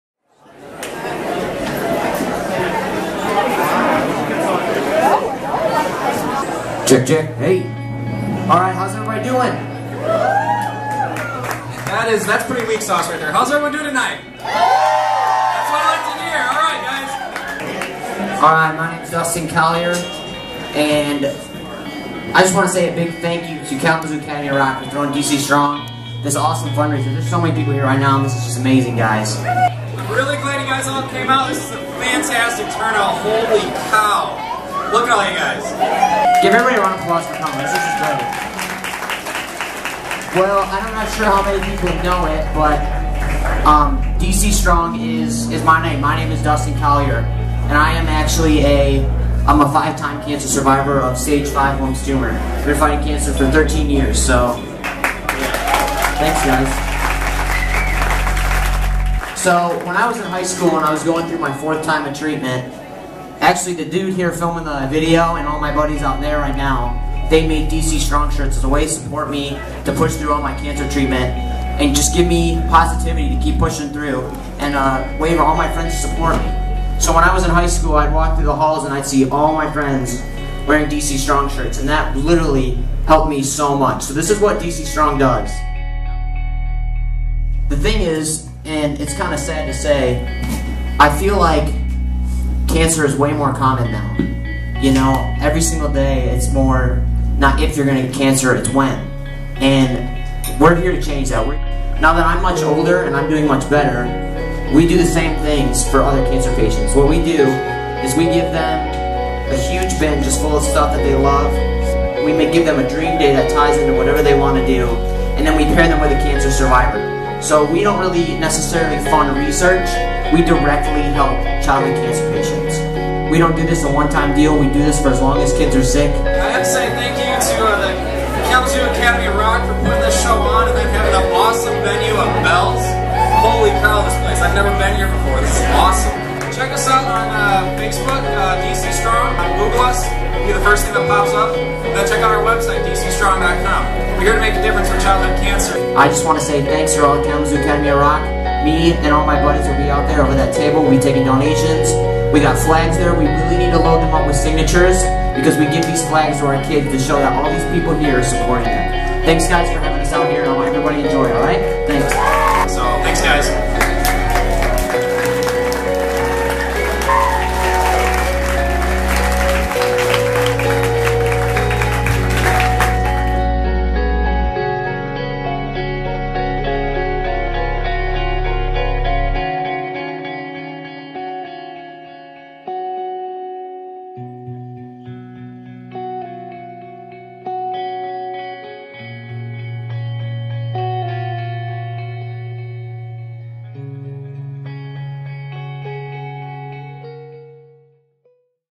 Kalamazoo Academy of Rock: DC Strong Benefit Concert
President Nate Smith & Vice-President Dustan Colyer give a speech to the hundreds of supporters at BELL'S Eccentric Café during the Kalamazoo Academy of Rock: DC Strong Benefit Concert.

Facebook.com/dustancolyerstrong